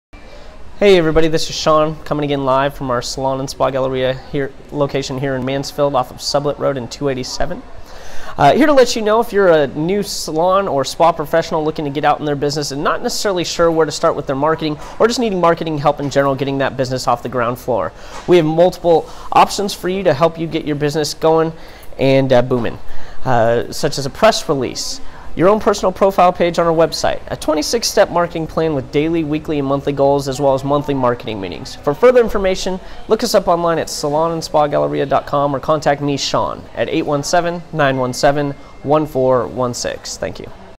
Marketing Materials with Your Salon Suite
One of the many amenities you receive when renting a salon suite at Salon and Spa Galleria is a press release, your own personal web page on our website, and monthly marketing materials!